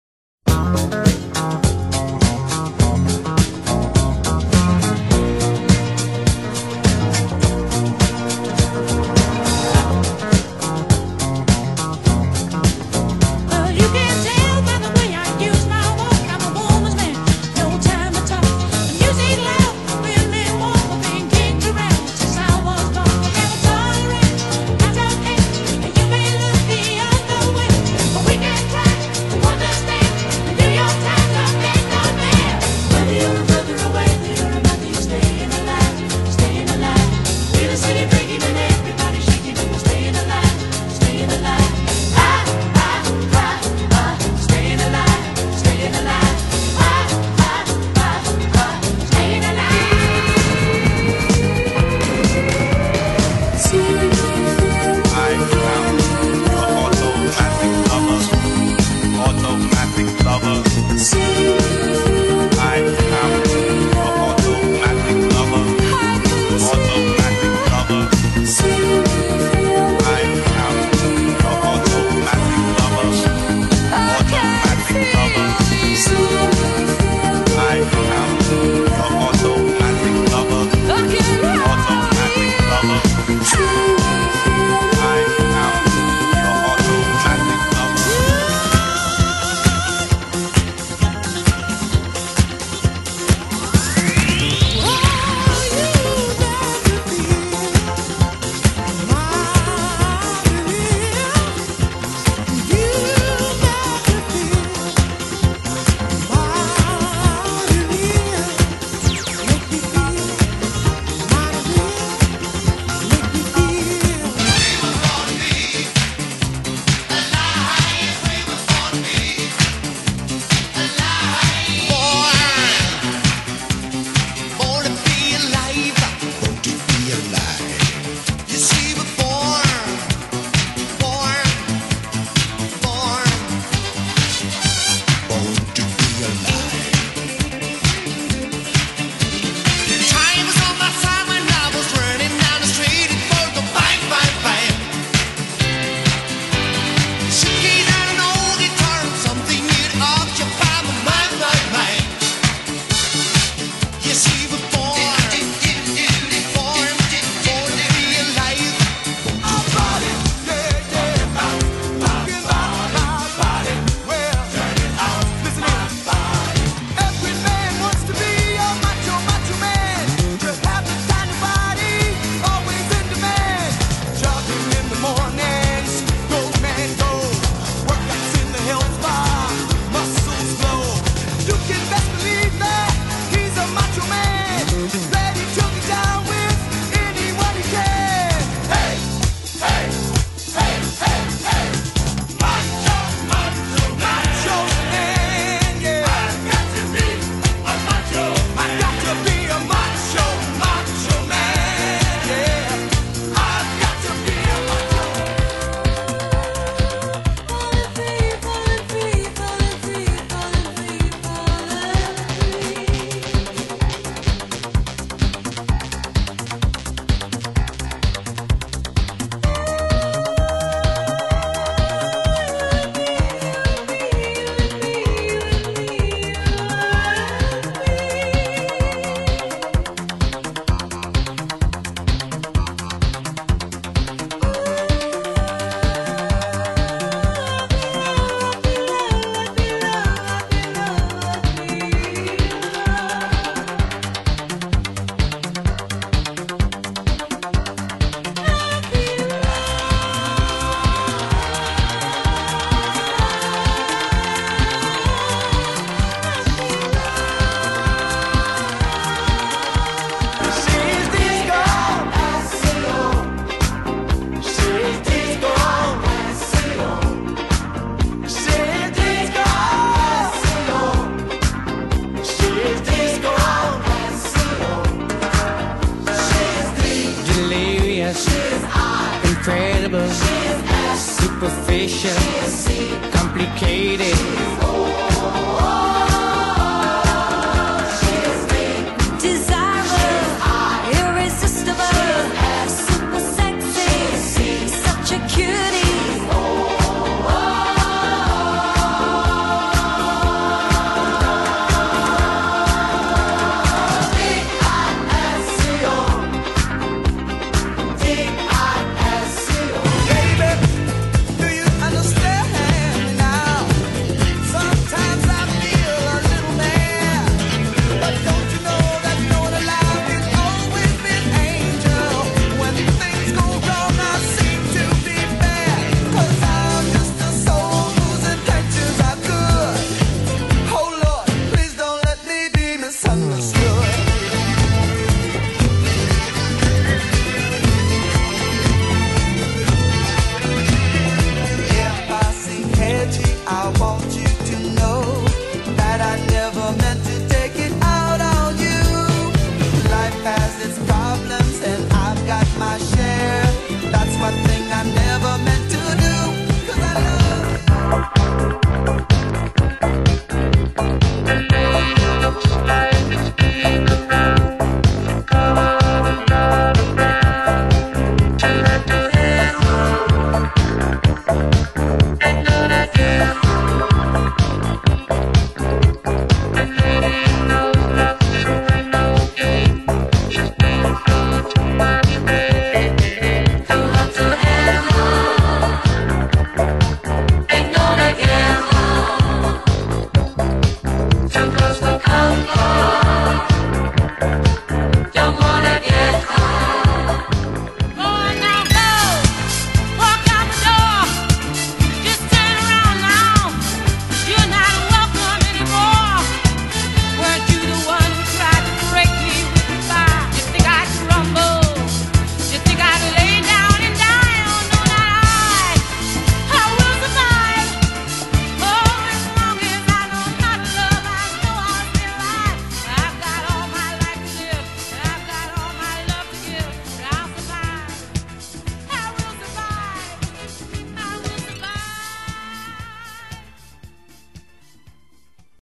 10 GRANDES SUCESSOS - DISCOTECA 70
No final dos anos 70,  a Discoteca se popularizou em todo o mundo com o filme que John Travolta era o protagonista "Os Embalos de Sábado a Noite", aqui no Brasil o auge foi com a novela "Dancing Days". Nesse slide show, dez musicas com nomes e fotos dos artistas da época.